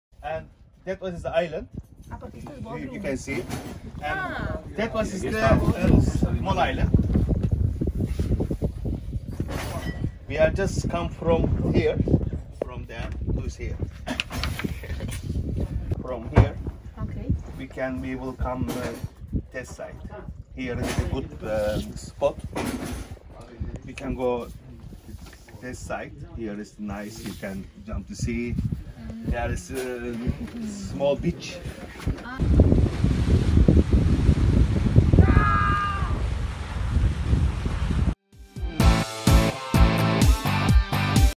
Vacaciones Goleta Explicacion ruta
Cuando alquilas una de nuestras goletas , todas las mañana el capitán explicara la ruta que se hará cada día y preguntara si os parece bien o que os apetece hacer. En este caso es la Goleta Kavanca disponible para alquilar desde Bodrum.